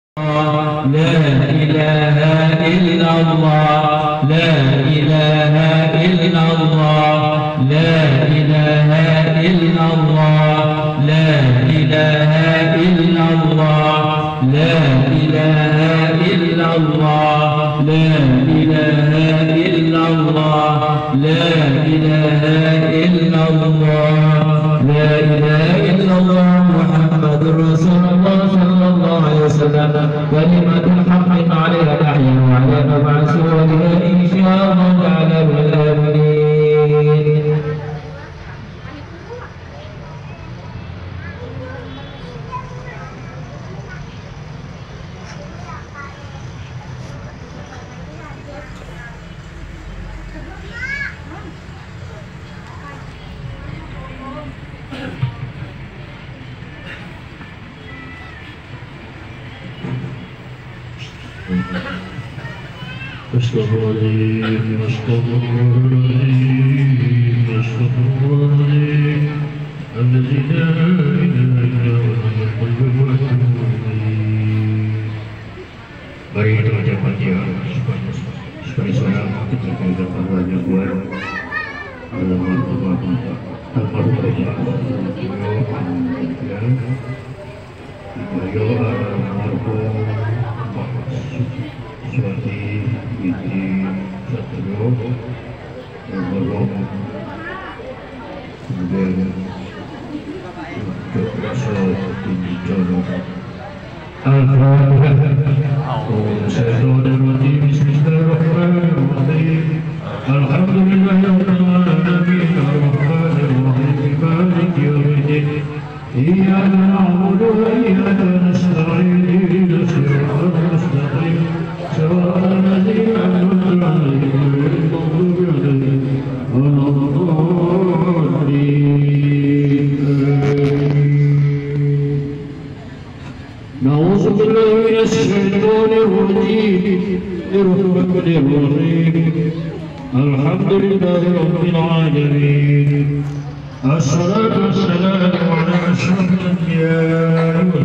BAGUS JUGA TAHLIL TANPA GELENGAN KEPALA DI PEKANBARU. RIAU INDONESIA.,
TAHLILAN SUDAH MEMBUDAYA, BISA TANPA GELENGAN KEPALA..,REKAMAN M,RAKIB JAMARI.